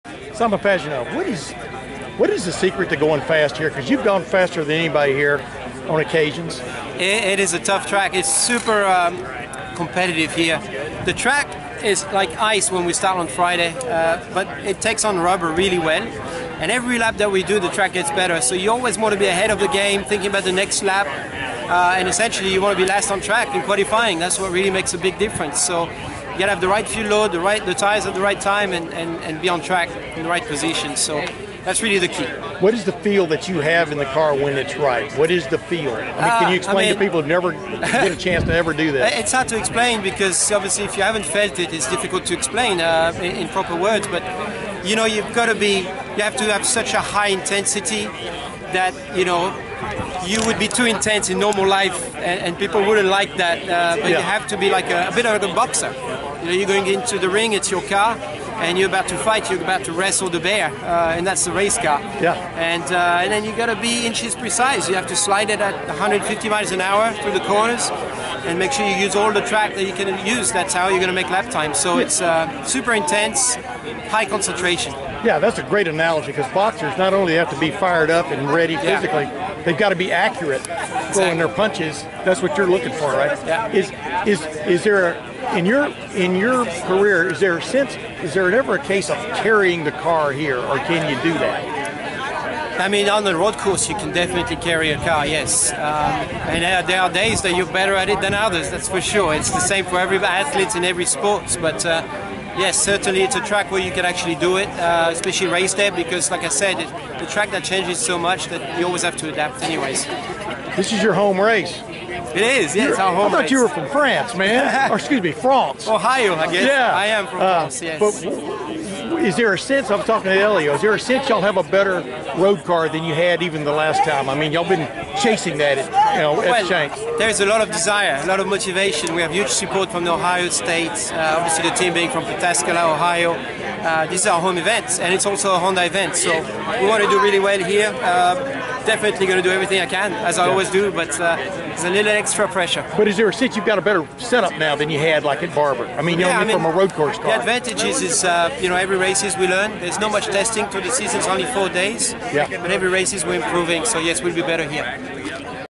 Simon Pagenaud on what "fast" means at Mid-Ohio - He has IndyCar record
Simon Pagenaud set the Mid-Ohio Sports Car Course IndyCar lap record of 63.8700 seconds in 2016, the same year he won the Honda Indy 200 at the track and eventually the season title. Now with Meyer Shank Racing for its "home race" this weekend, the Frenchman explains what it takes to go fast, and then maybe get a little extra for qualifying.